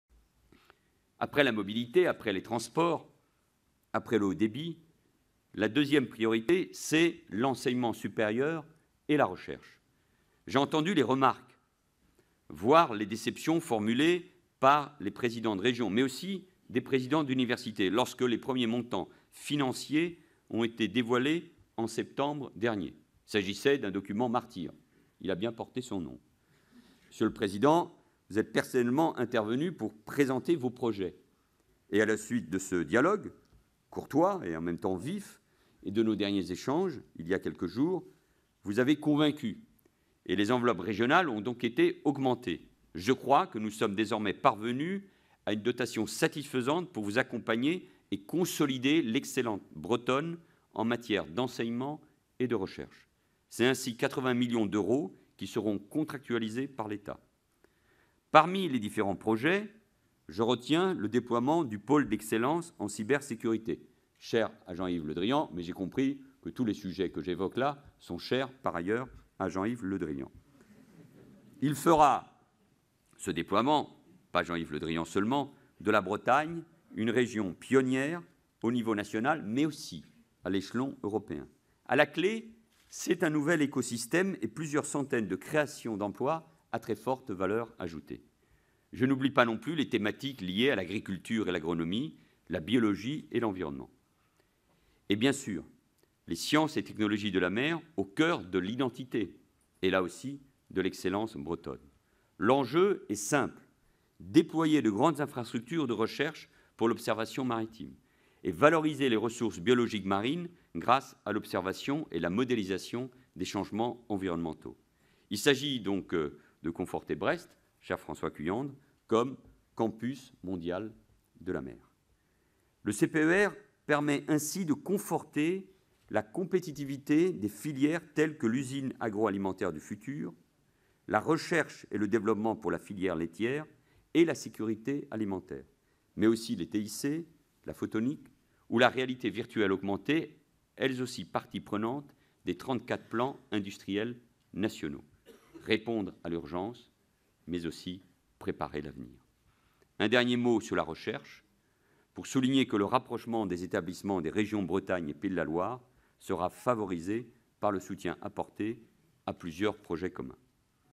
La seconde priorité du CPER : l'enseignement supérieur et la recherche
Dans son discours à Océanopolis devant les acteurs économiques et sociaux, Manuel Valls, le Premier ministre précise les enjeux du CPER (Contrat de Plan Etat-Région).
La seconde priorité : l'enseignement supérieur et la recherche